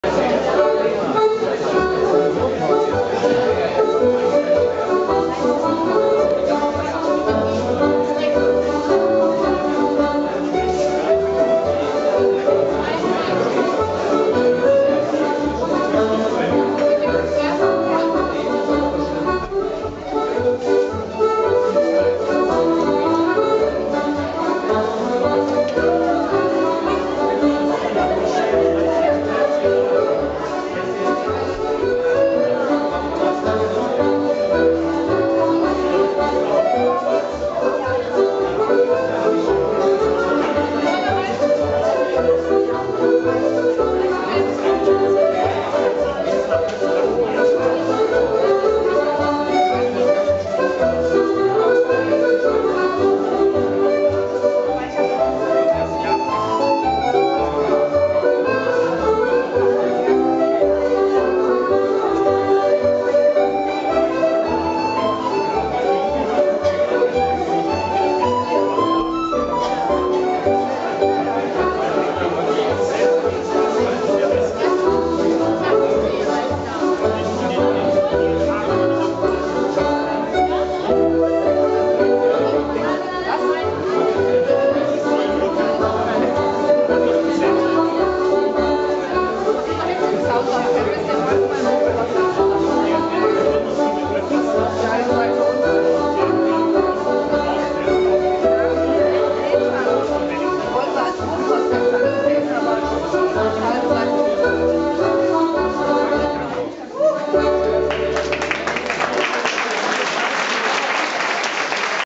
Yvonne's Lasses - J96 4c sq. - dev. by Roy Goldring (Gr. + Social Dances 3)
recorded on the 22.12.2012 at the 20th Christmas Ball in Münster/Germany